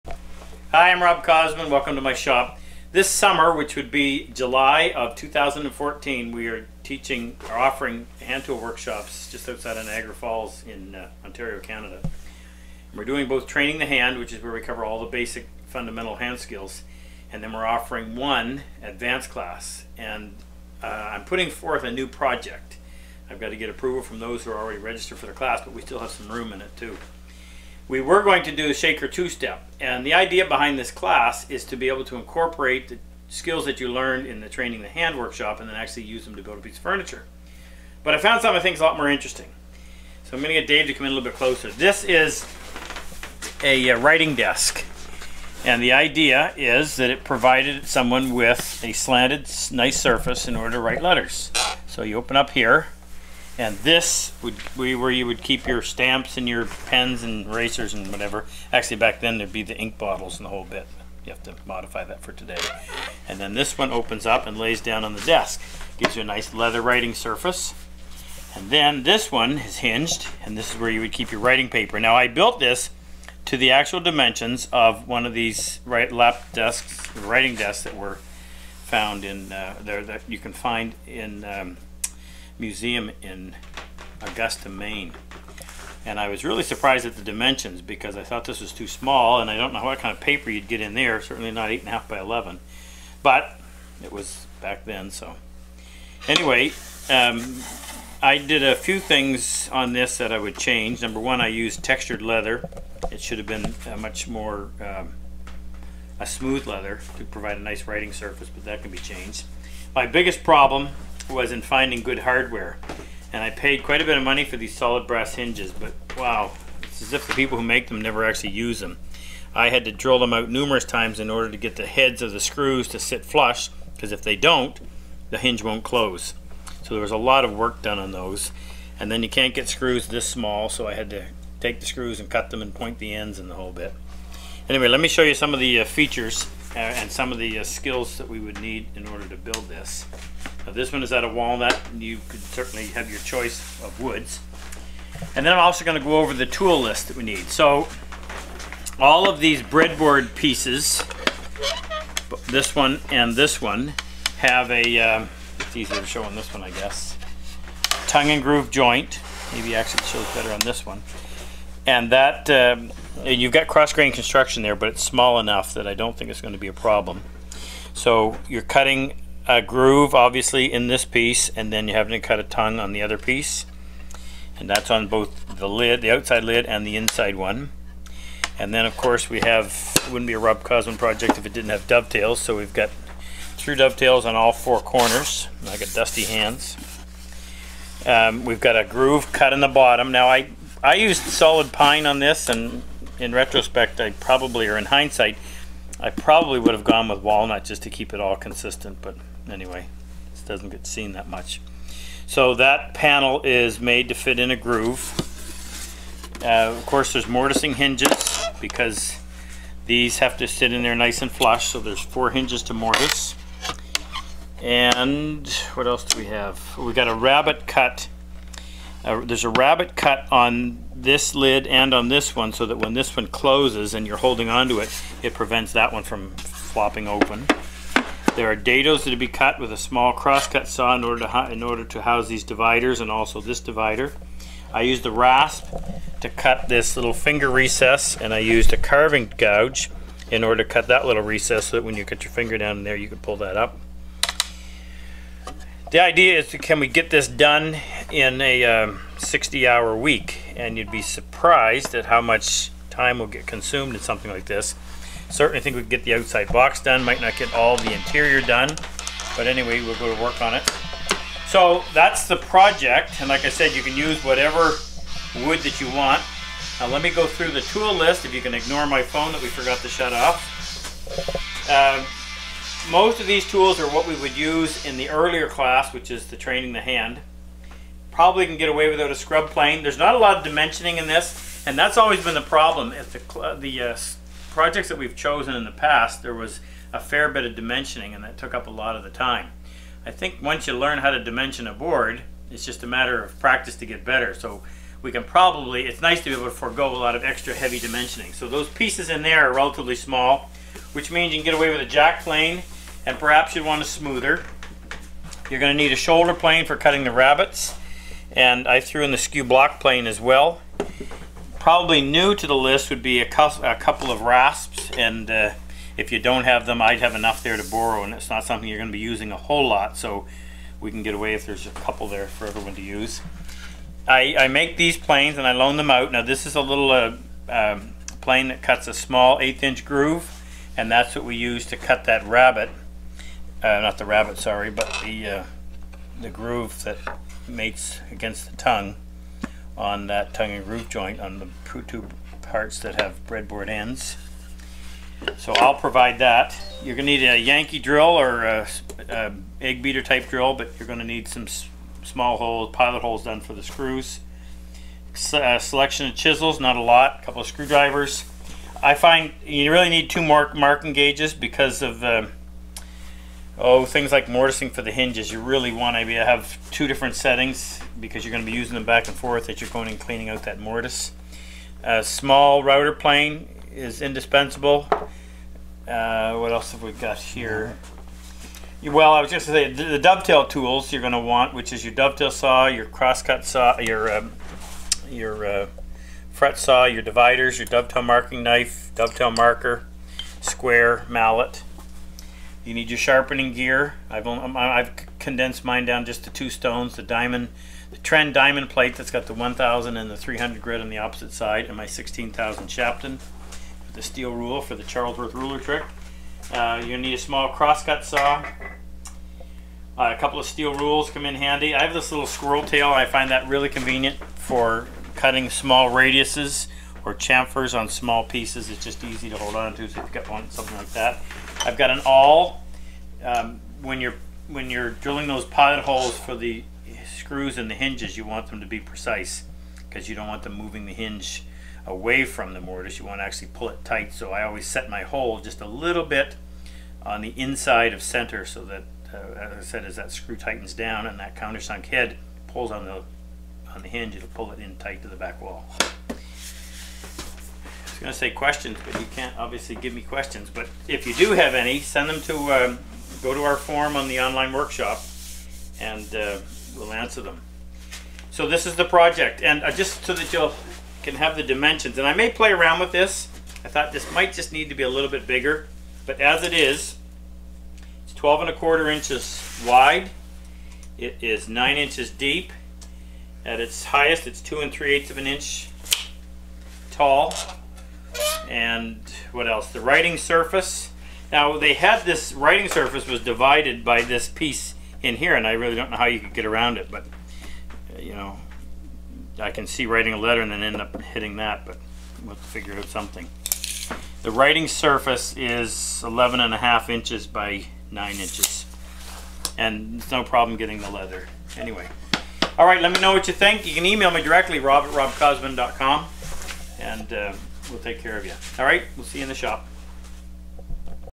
Rob Cosman's Advanced Hand Tool Workshop, 2014
This is the project I am proposing for the summer of 2014, July 14-18.